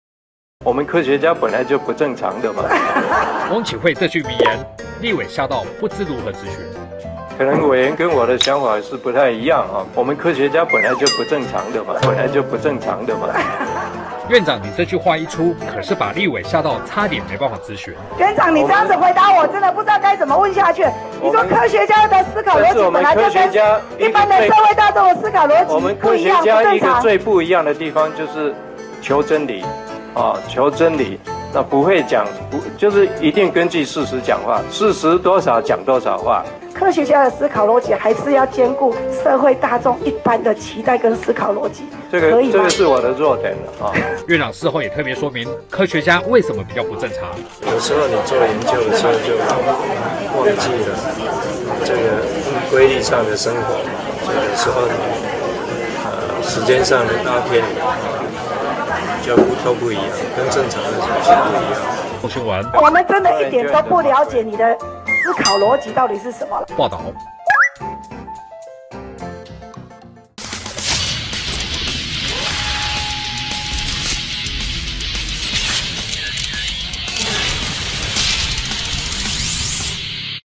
翁啟惠這句名言 立委嚇到不知如何質詢--蘋果日報20160428
立委何欣純昨天在立法院教育文化委員會質詢時表示，中研院長翁啟惠的想法可能跟全台灣民眾都不一樣，脫離社會期待，結果翁啟惠竟回應，「科學家本來就不正常」，這句話可把立委何欣純嚇到差點無法質詢。